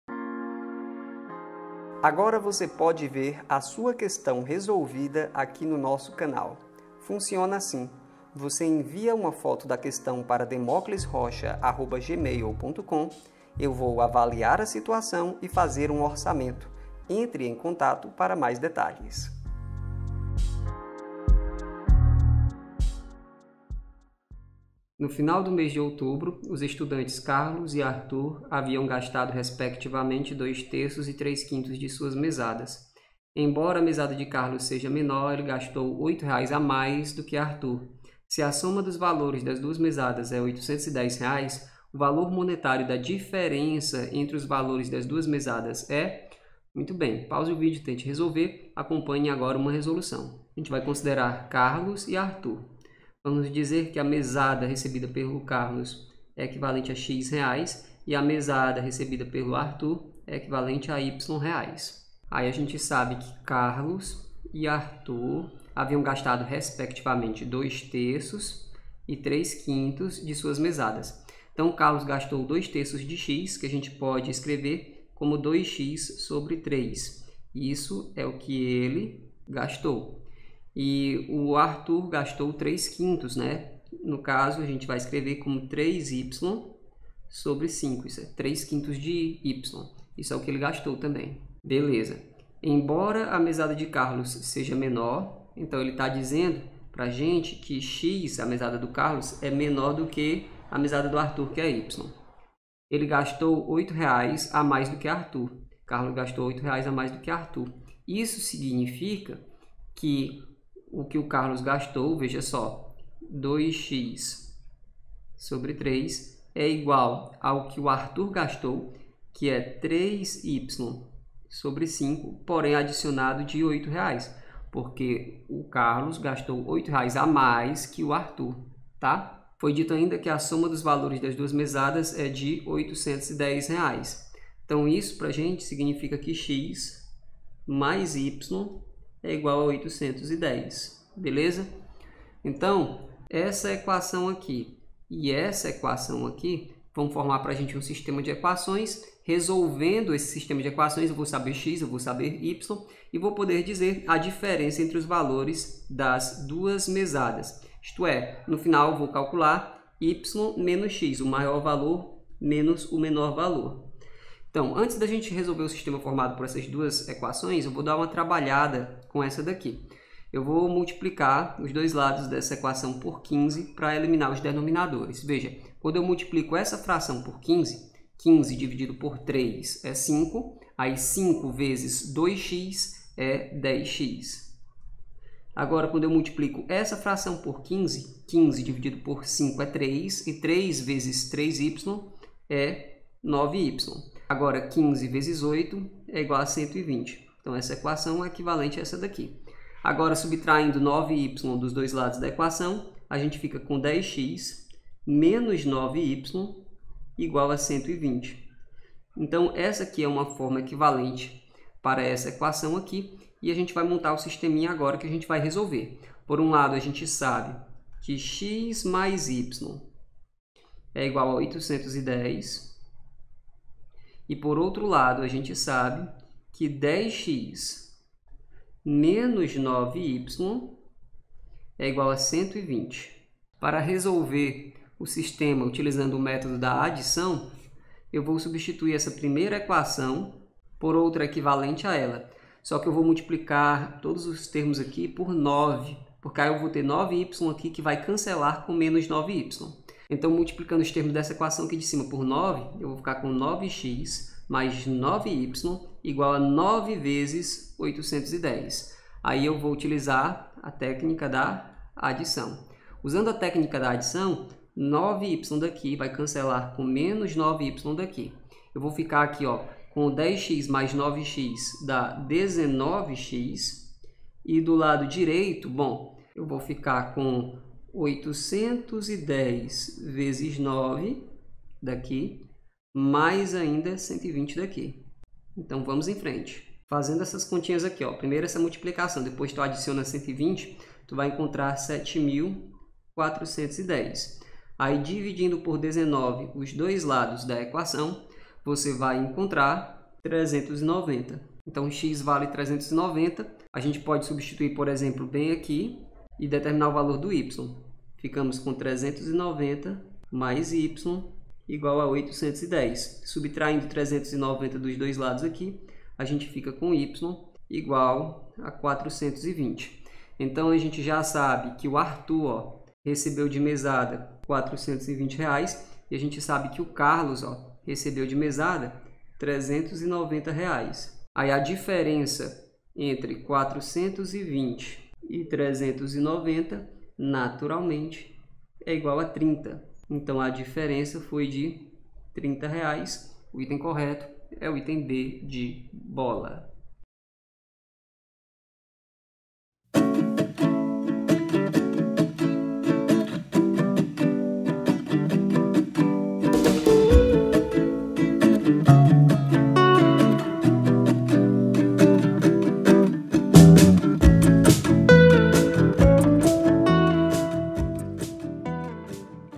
PROBLEMA ENVOLVENDO EQUAÇÕES
(UECE 2015.1 2a Fase) No final do mês de outubro, os estudantes Carlos e Artur haviam gastado respectivamente dois terços e três quintos de suas mesadas. Embora a mesada de Carlos seja menor, ele gastou R$ 8,00 a mais do que Artur. Se a soma dos valores das duas mesadas é R$ 810,00, o valor monetário da diferença entre os valores das duas mesadas é